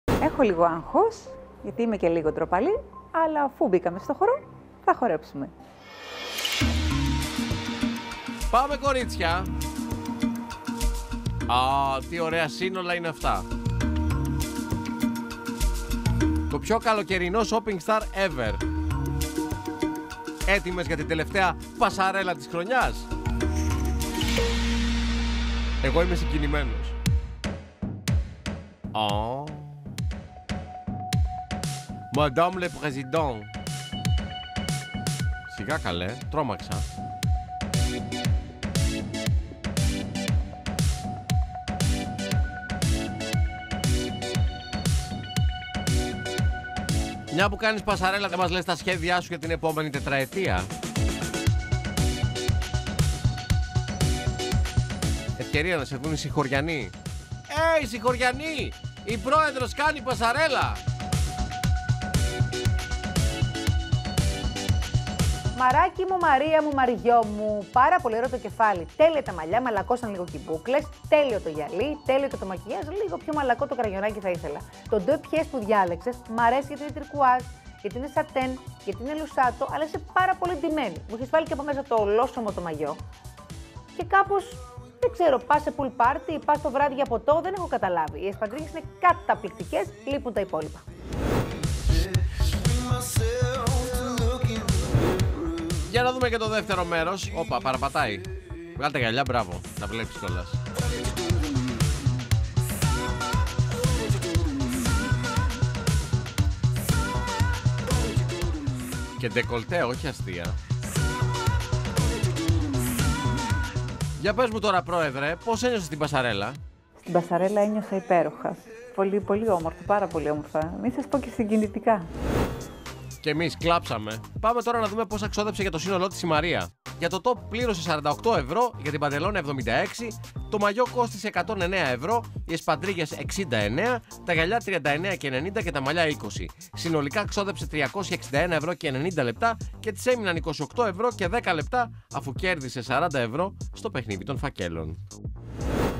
Catwalk Μαρίας «Pool party σε 5αστερο ξενοδοχείο»
Δείτε τις επιλογές σε ρούχα και παπούτσια της Μαρίας για το θέμα της εβδομάδας «Pool party σε 5αστερο ξενοδοχείο»
Καθημερινά, στις 16:00.